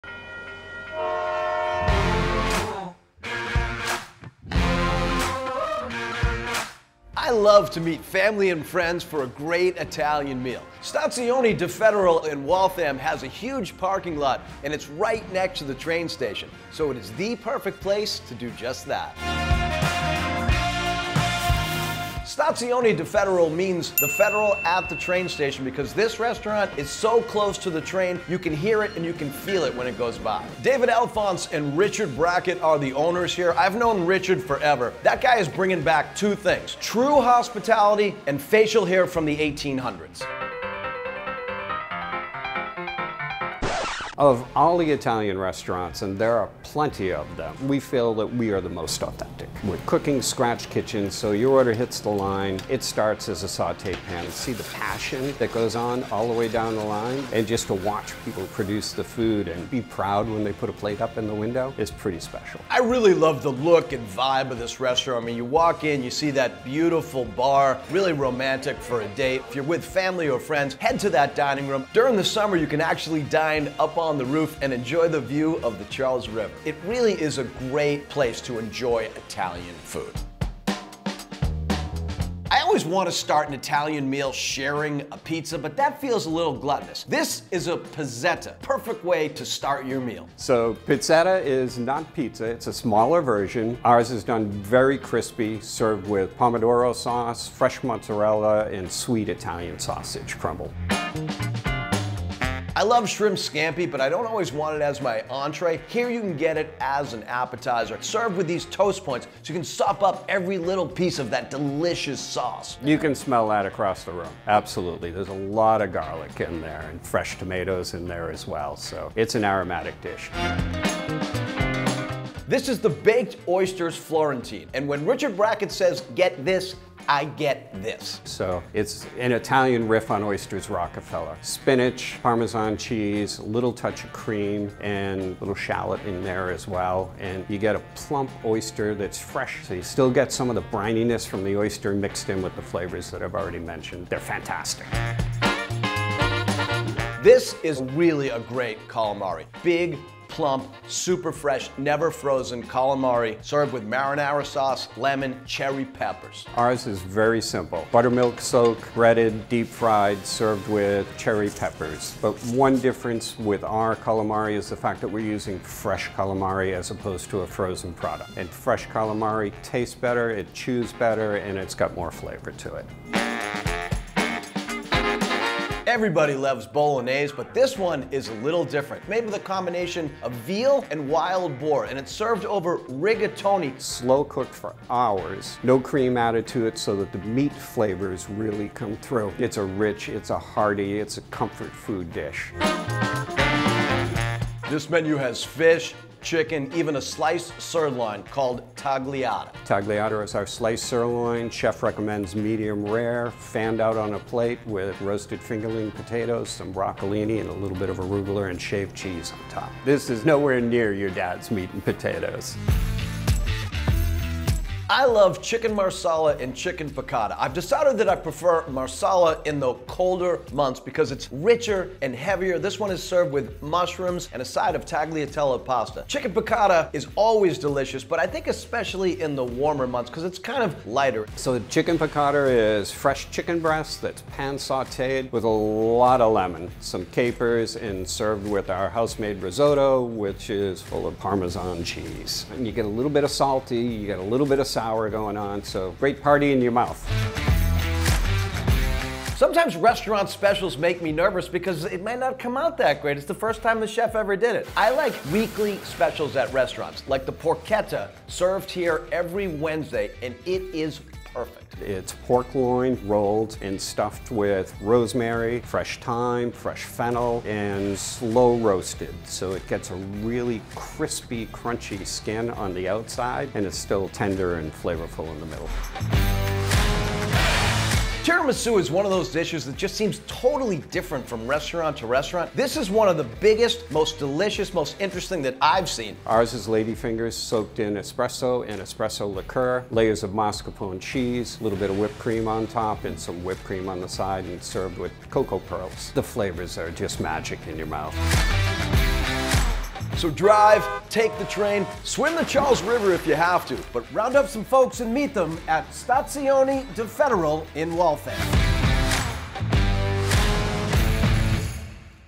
Stazione di Federal in Waltham
I love to meet family and friends for a great Italian meal.  Stazione di Federal in Waltham has a huge parking lot and it's right next to the train station. So it is the perfect place to do just that.

 Stazione di Federal means ‘the federal at the train station' because this restaurant is so close to the train, you can hear it and you can feel it when it goes by.